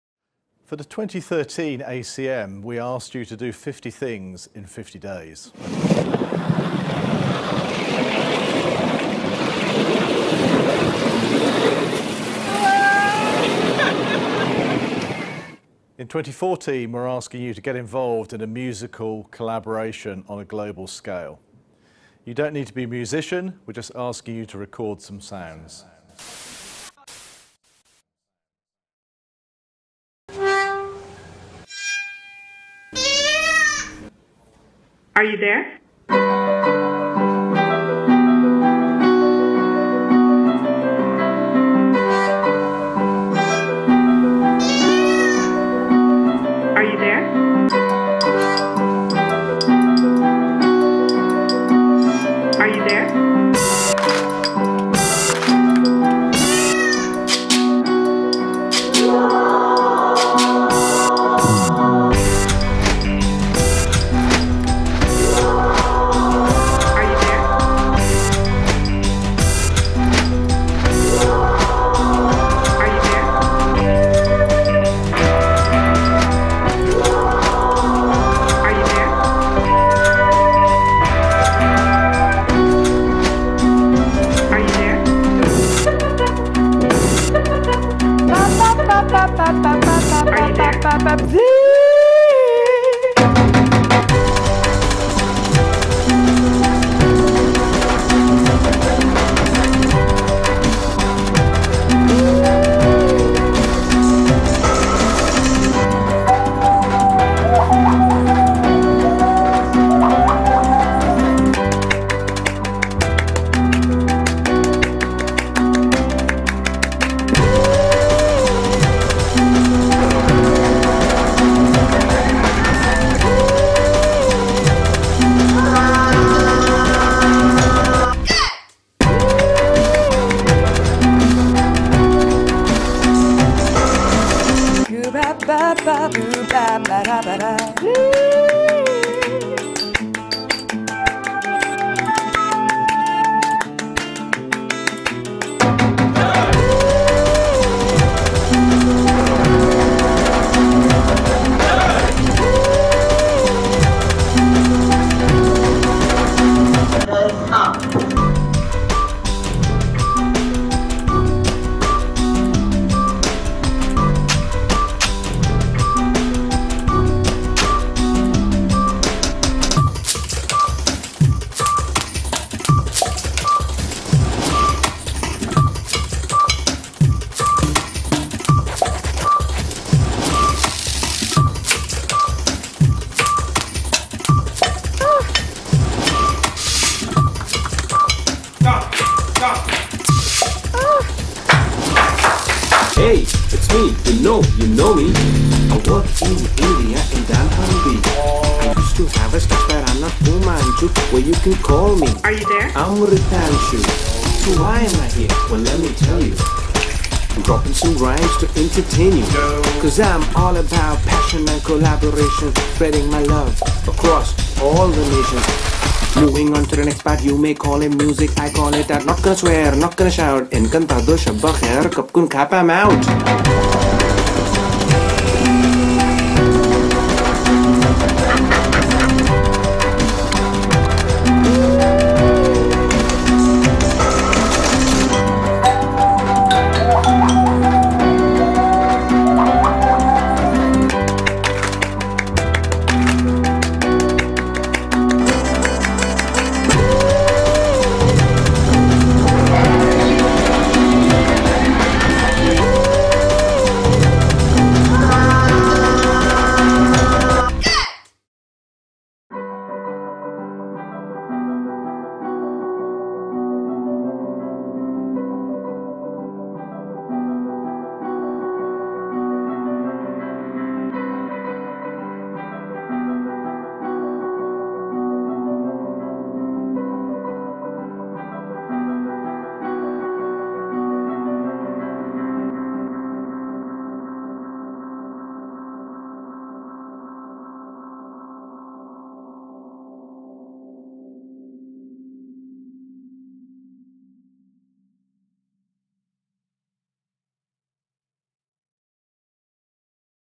The sound of dunnhumby
We asked employees to make a sound, musical or otherwise and film it. Three months and a lot of editing later, this is the result...